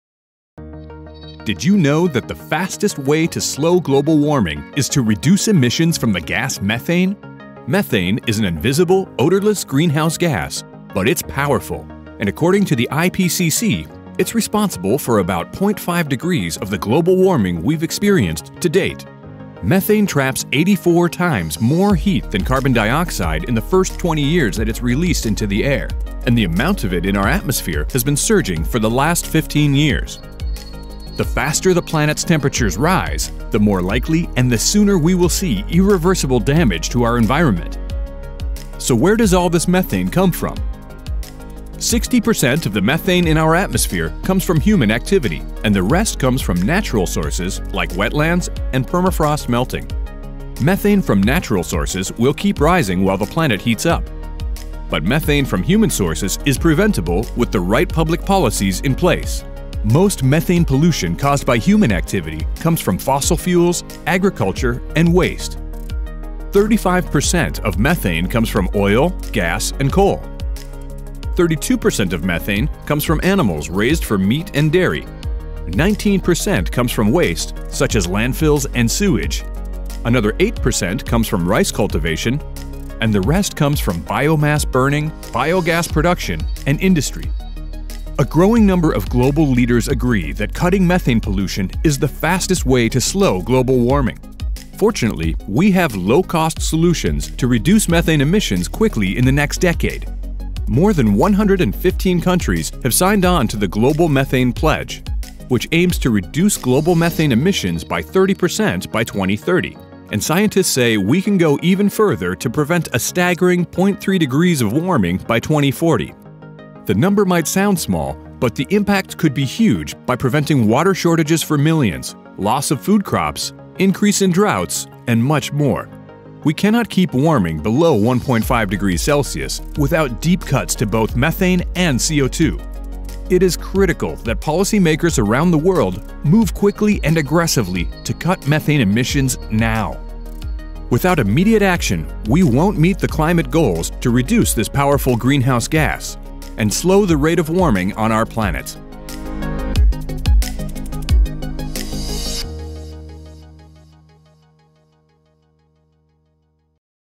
What is methane pollution?
Methane is a harmful super pollutant that warms the planet more than 80 times more than carbon dioxide over its first 20 years in the atmosphere. It’s responsible for about half a degree of warming so far, and its levels are rising fast – the atmospheric growth rate in 2021 was the fastest in years. Due to its short-lived nature, however, there are significant short-term benefits to cutting methane emissions now. In fact, reducing methane pollution is the fastest way to slow global warming and avoid near-term and irreversible impacts such as collapsing glaciers.